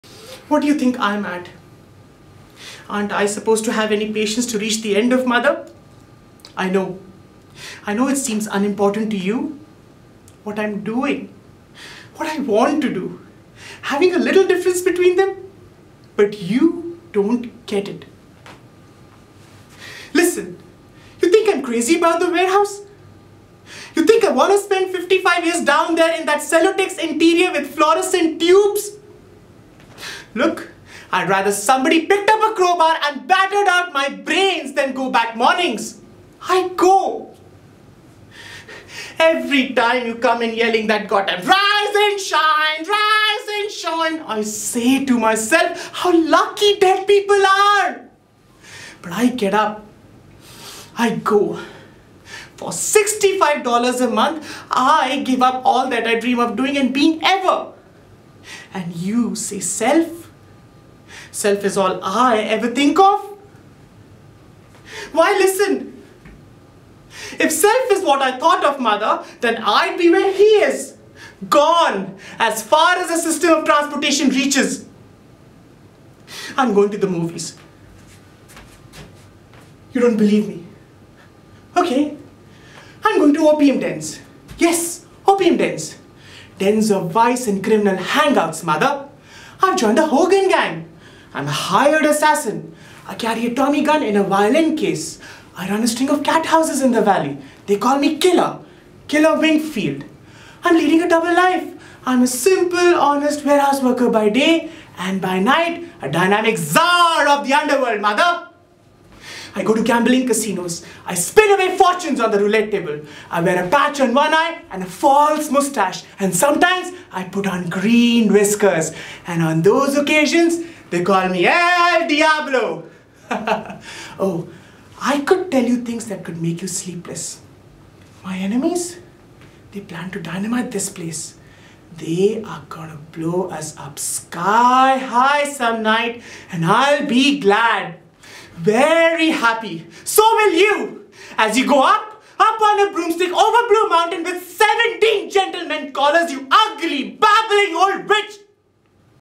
Tennessee William | The Glass Menagerie | Tom Wingfield
This is a monologue from Tennessee William's Memory Play "The Glass Menagerie". Here Tom is confronting his mother Amanda Wingfield who tells him that she has reached the end of her patience.
Location:  Dubai, United Arab Emirates (UAE).

Special Thanks:
World Monologue Games 2020 (WMG)
The Monologue Project